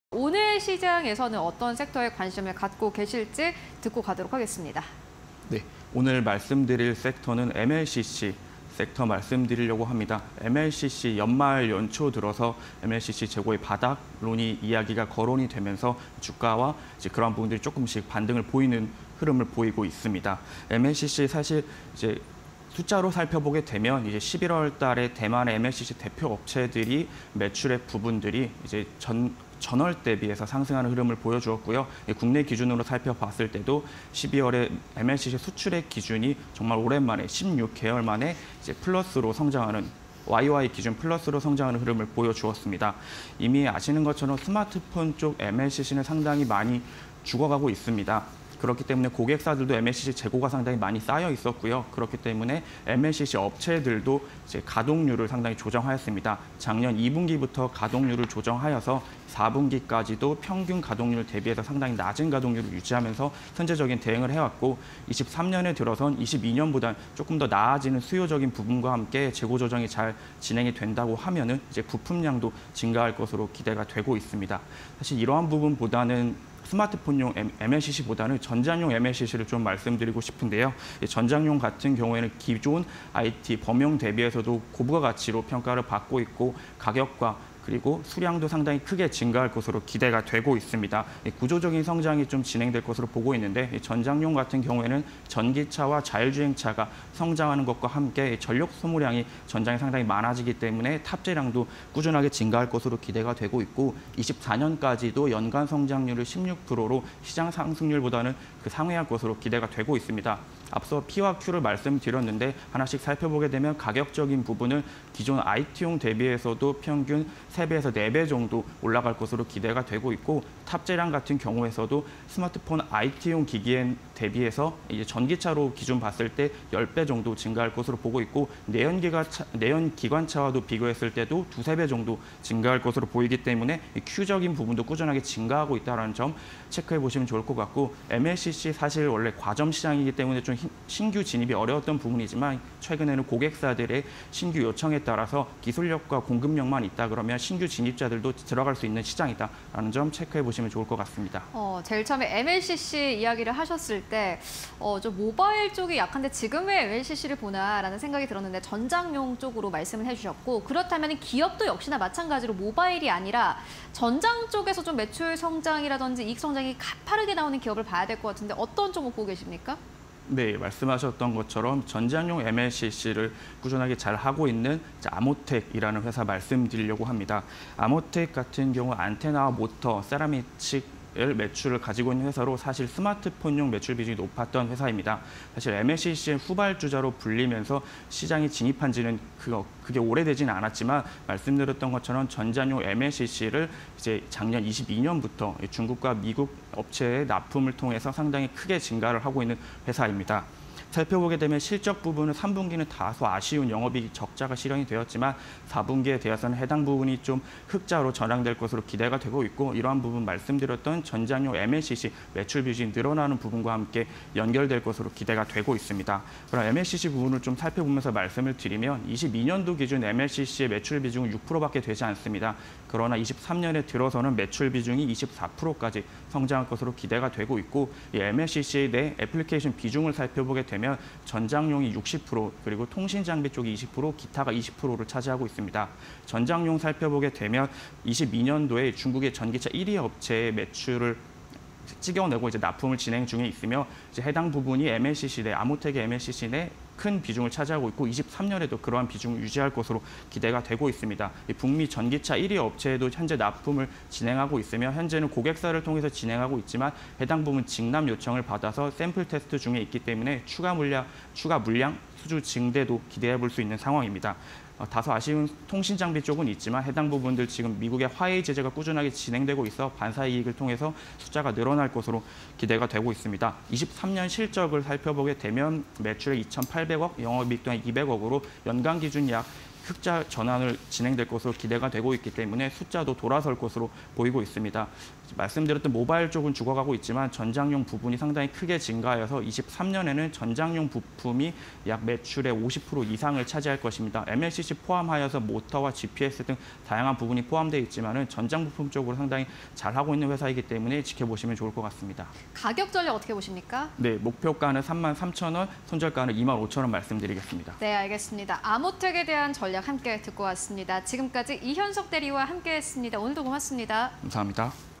아모텍(052710)... MLCC(적층세라믹캐패시터)_시초가에 이 종목 (20230131)
- MLCC 업황, 올해 1분기 바닥 시그널 전망: 지난해 12월 국내 MLCC 수출액 성장률, 16개월만에 플러스 전환
- 글로벌 MLCC 업체 가동률 낮은 편: 지속적 재고 조정 및 세트 출하량 점진적 반등
- 글로벌 MLCC 업체의 정상 재고 시점 주목
- 올해 2분기 정상 재고 복귀 전망 多
- 中 스마트폰 수요 둔화 원인 '락다운' 리스크 해소 중
- 전장용 MLCC 성장세
- 안정적 공급 물량 위해 고객사 다변화 추세
- 기술 진입 장벽 높아… 역량 중요
- 기존 납품 레퍼런스 중요

아모텍(052710)
- 전장용 MLCC 매출 본격화
- MLCC 매출 비중, 올해 24% 성장 전망
- 중국·북미 전기차 업체향 MLCC 공급 중
- 영업익 216억원으로 흑자 전환 전망